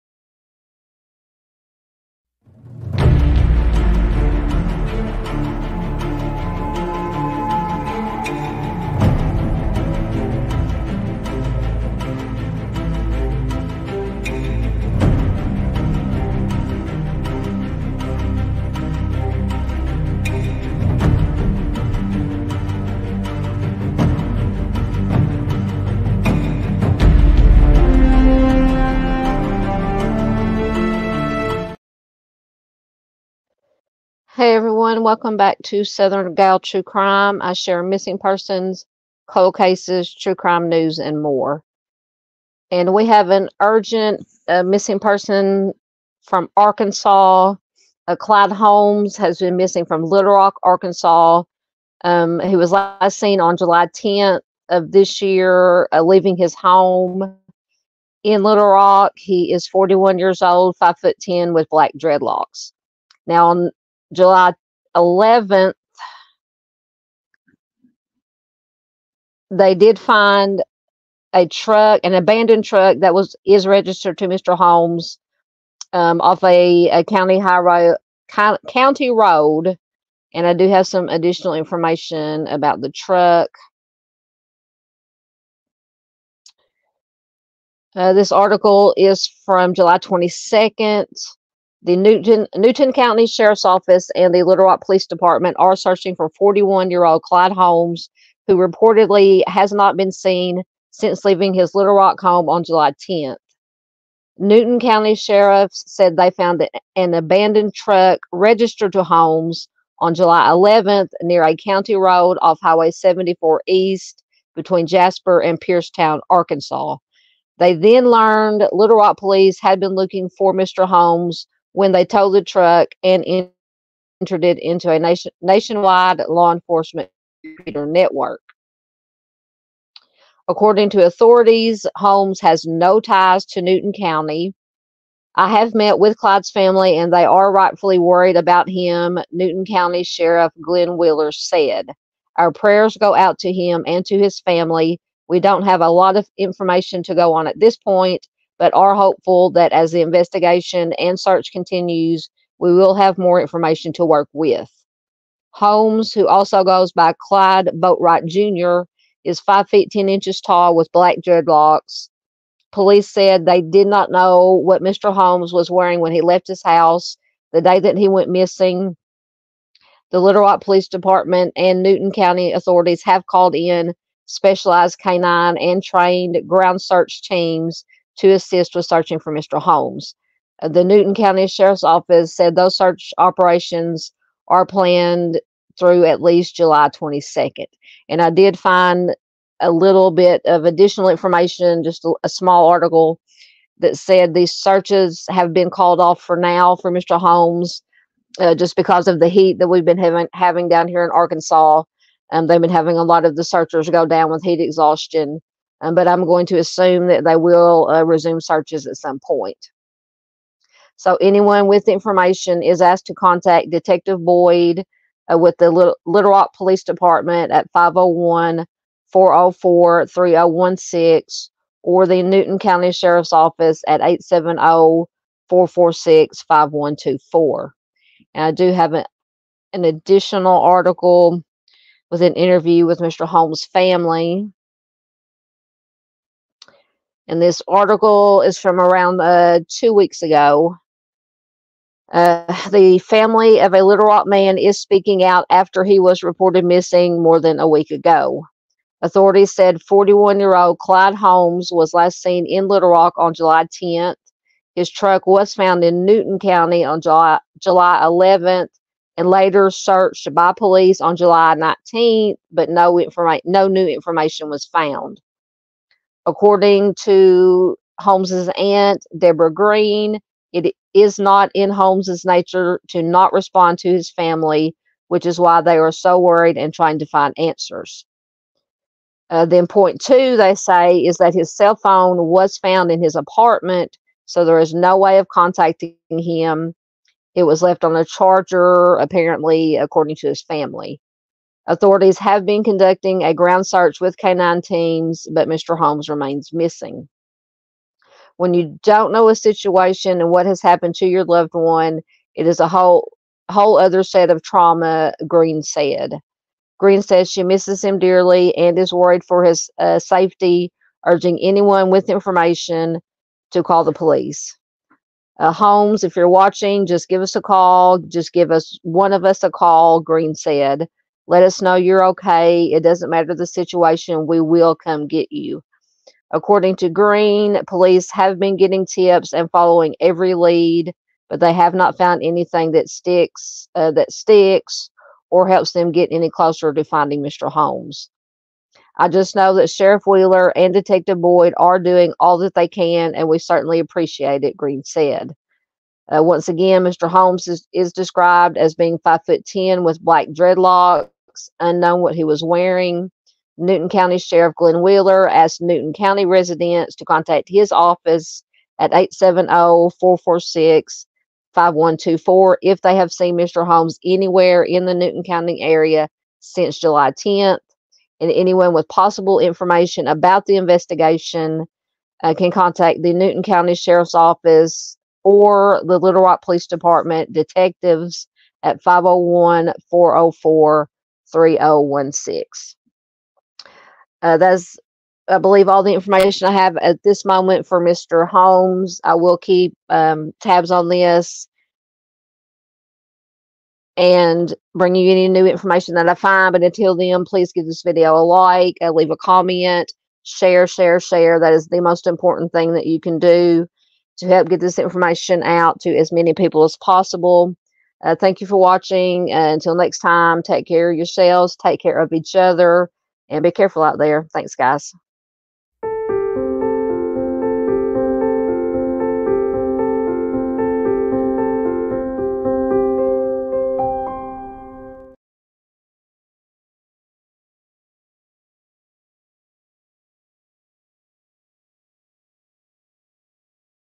MISSING: Clyde Holmes, Little Rock, AR
📍LITTLE ROCK, Ark. — The Newton County Sheriff's Office and the Little Rock Police Department are searching for 41-year-old Clyde Holmes, who reportedly hasn't been seen since leaving his Little Rock home on July 10...

ℹ️Sources
🌟THV11.com-